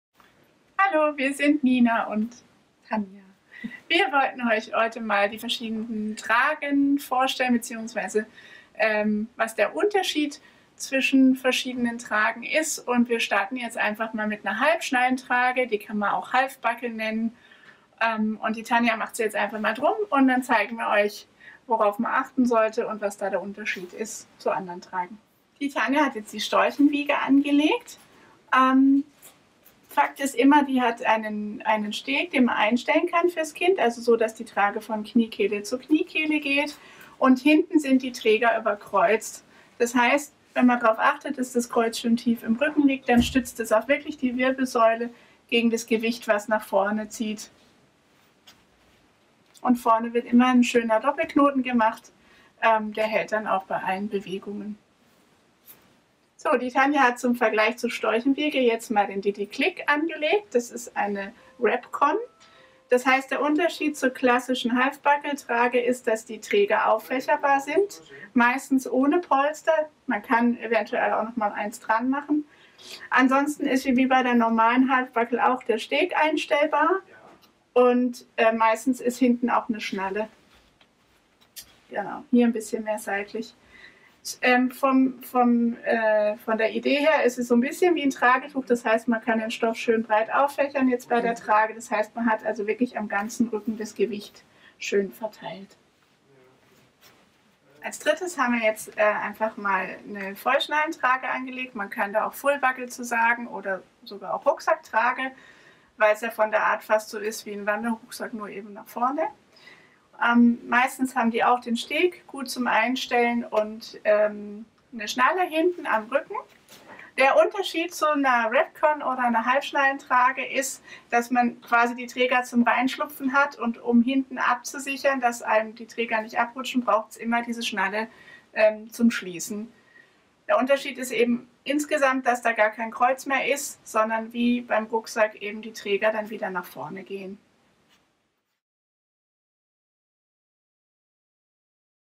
3 unterschiedliche Babytragen: Fullbuckle, Halfbuckle und WrapCon
Bei Babytragen unterscheidet man grob 3 Modelle:
Halfbuckle - am Hüftgurt gibt es eine Schnalle, die Schulterträger werden gebunden.
WrapCon - Hüftgurt mit Schnalle oder zum Binden, die Schulterträger sind auffächerbar und zum Binden.
Fullbuckle - alle Gurte werden mit Schnallen geschlossen.

Es gibt natürlich noch andere Begriffe und Modelle von Babytragen, doch das sind bei uns im Laden in der Trageberatung die beliebtesten. Deshalb stellen wir Dir diese 3 vor. Tanja hat jeweils eine Babytrage angelegt und Nina erzählt Dir die wichtigsten Fakten dazu.

Melde Dich gerne telefonisch, per email oder im Laden bei uns, falls Du Fragen zum Thema Babys tragen hast. Wir freuen uns auf Dich!